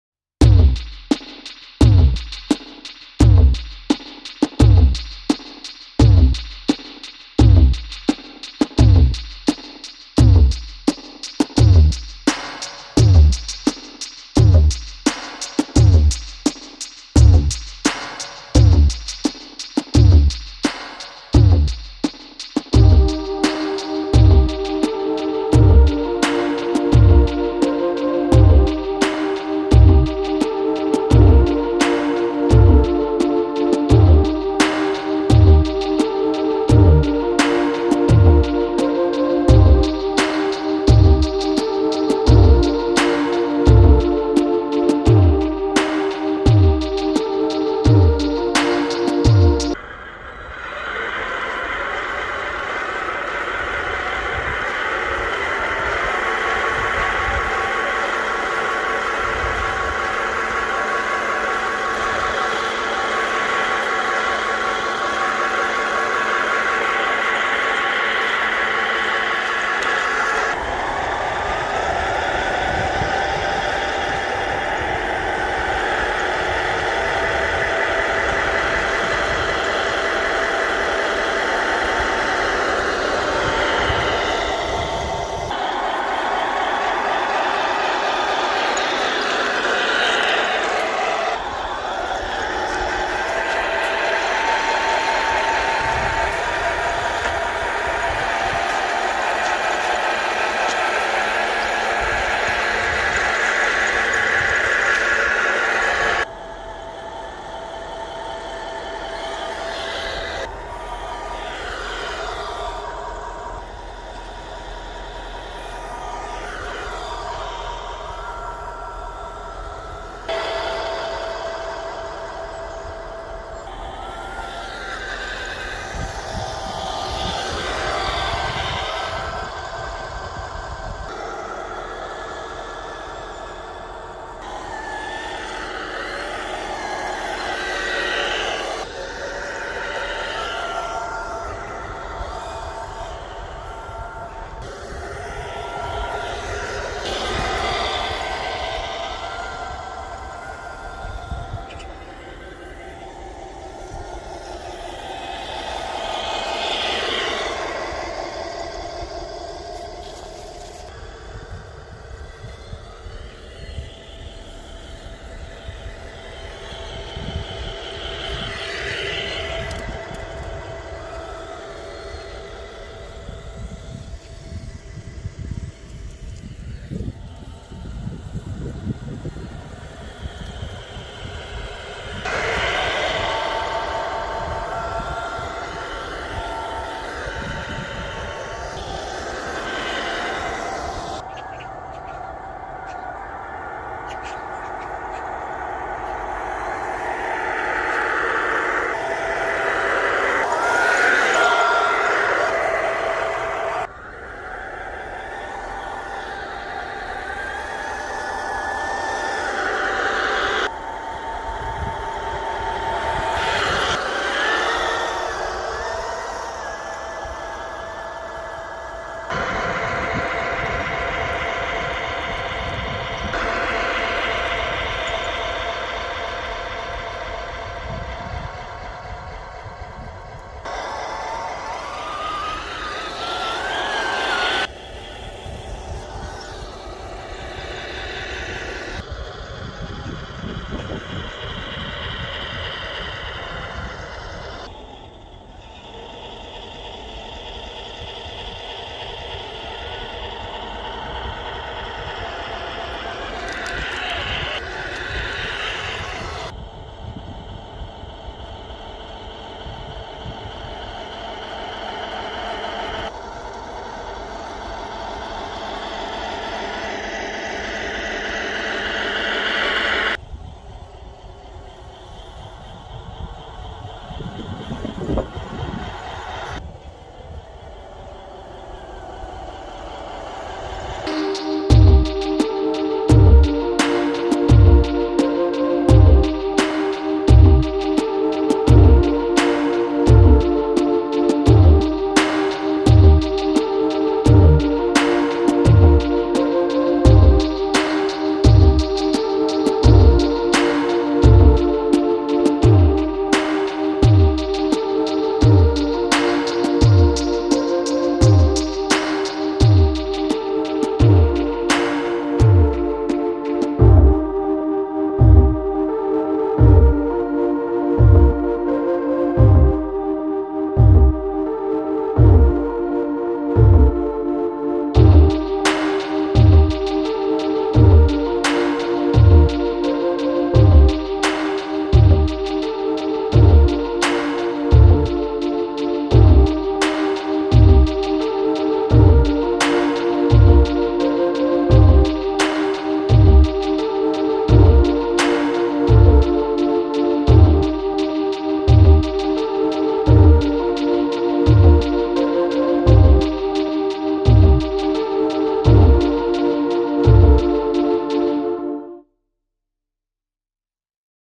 German Heavy Tank Tiger I, Moving to the front line Part 2 Zigzag testing on the snow field 모형 탱크 주행
1/16th Tamiya RC Tank

with ABER Upgrade Set
Just released from the factory and moving to the front line
Before camo painting and equipment installation